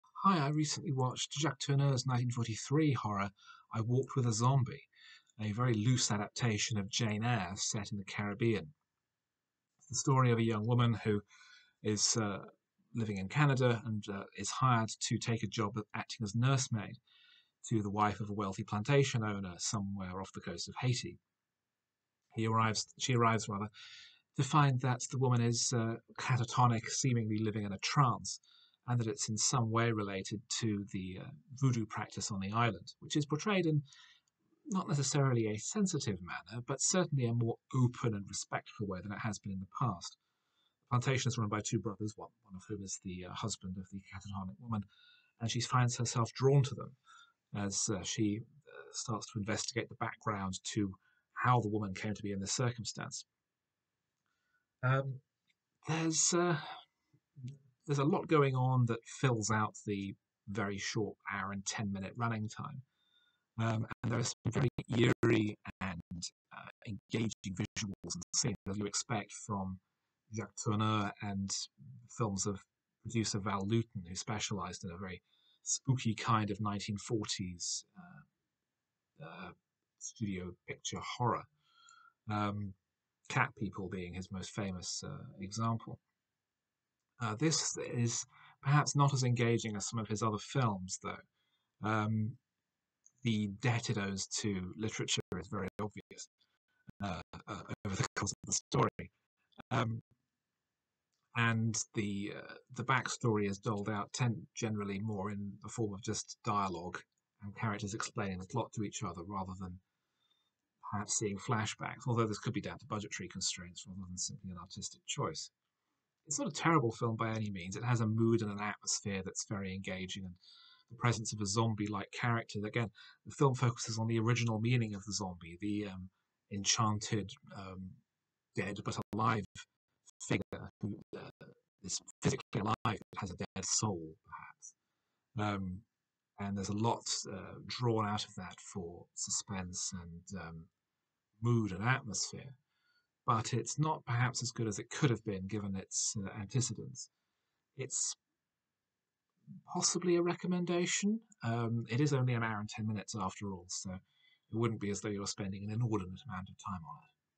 First Takes - I Walked with a Zombie (1943)
Jeremy watches the horror melodrama, directed by Jacques Tourneur and starring Frances Dee, Tom Conway, James Ellison and Edith Barrett.

Cinema Limbo is also a podcast.